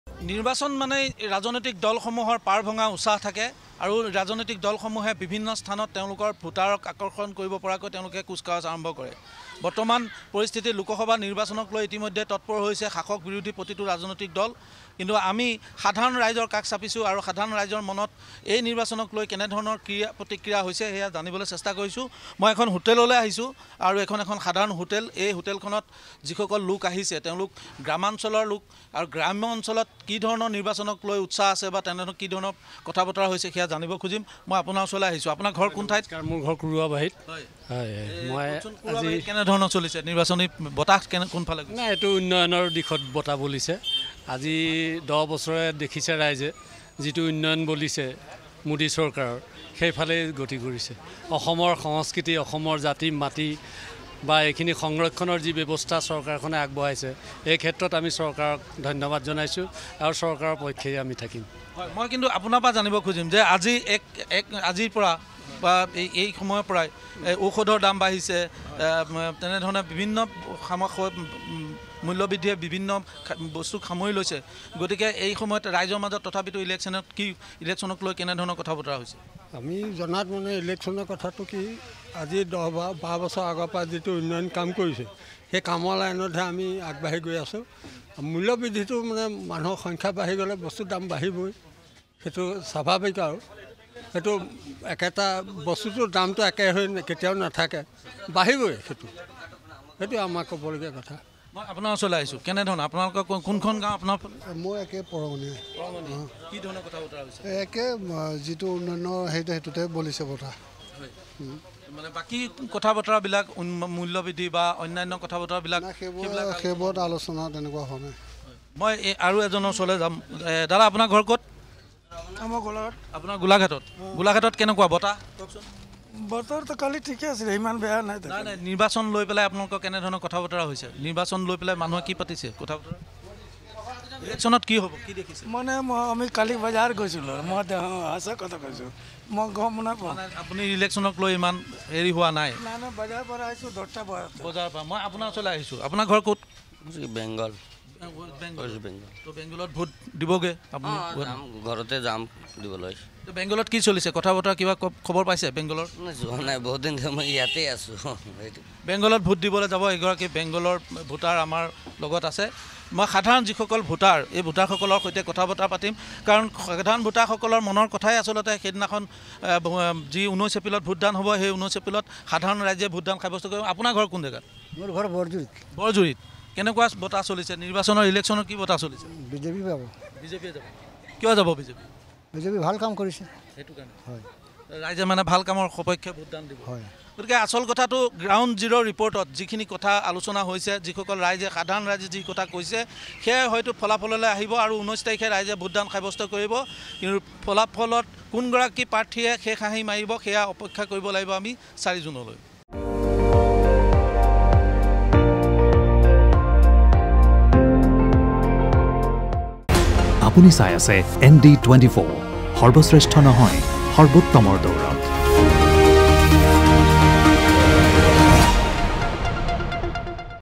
Lok Sabha Polls 2024: নিৰ্বাচনক লৈ কি কয় বোকাখাতৰ সাধাৰণ ভোটাৰে?
চমু চাপি আহিছে লোকসভা নিৰ্বাচনৰ দিন। নিৰ্বাচনক লৈ কি কয় বোকাখাতৰ সাধাৰণ ভোটাৰে?

▶️ ৰাজপাট কাৰ?